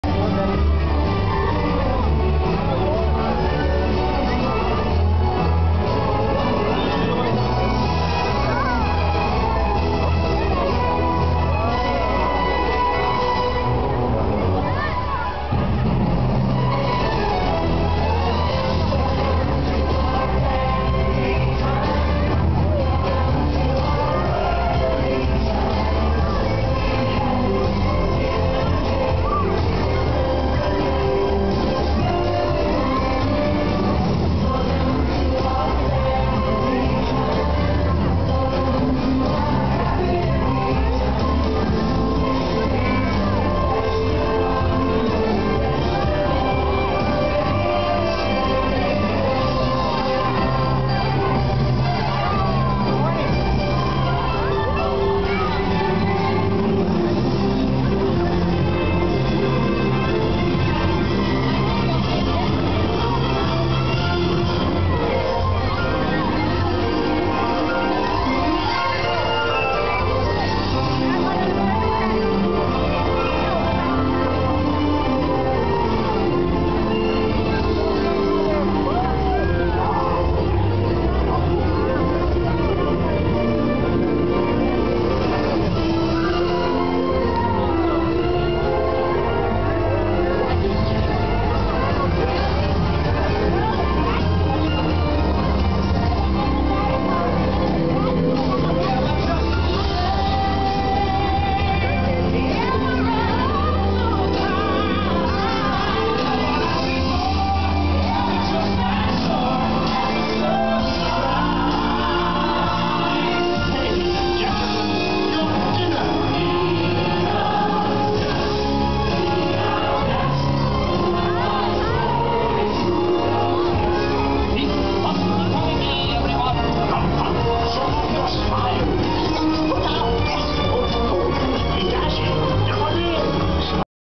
Parade of Dreams at Disneyland Night SWM CLIP 2 09/25/06
The Parade of Dreams at Disneyland, as seen along the East side, near the middle of 'Small World Mall' area, 2nd level, featuring parade floats 1 & 2, AKA "Gateway to Dreams" & "Dreams of Enchantment", at night, CLIP 2, filmed on 09/25/06